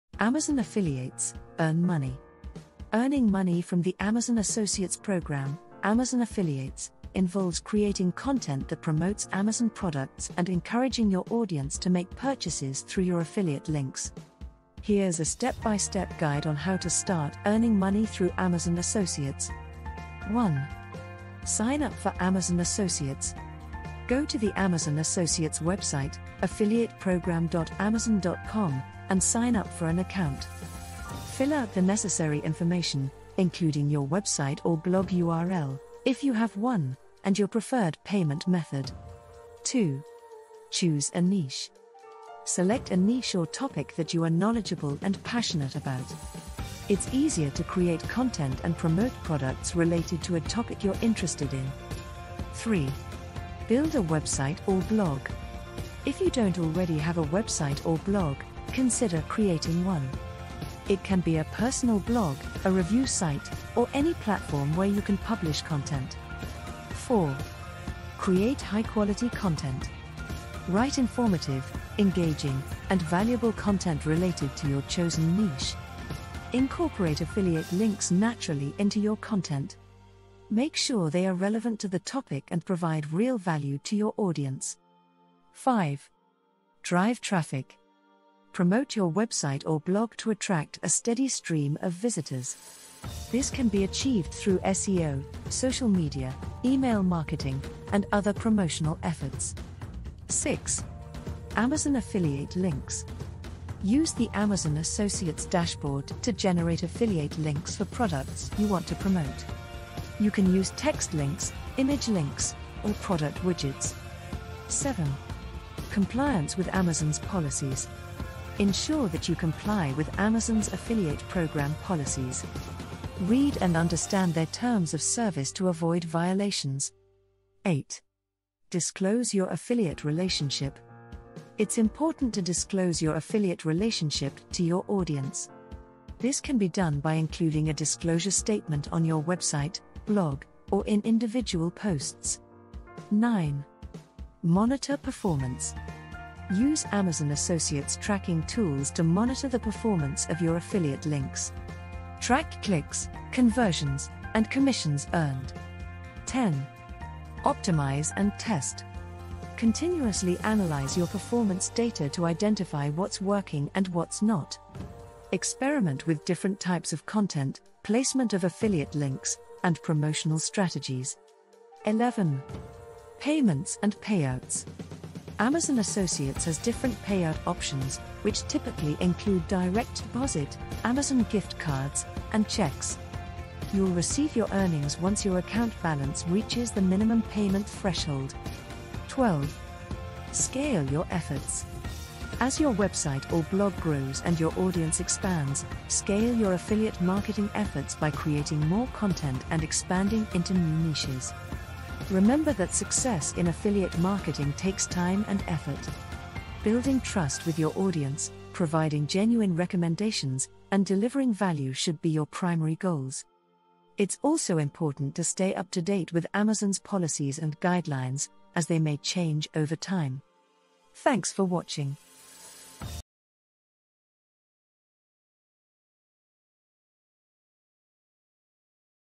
Amazon Affiliates: Earn Money
Earning money from the Amazon Associates program (Amazon Affiliates) involves creating content that promotes Amazon products and encouraging your audience to make purchases through your affiliate links. Here's a step-by-step guide on how to start earning money through Amazon Associates: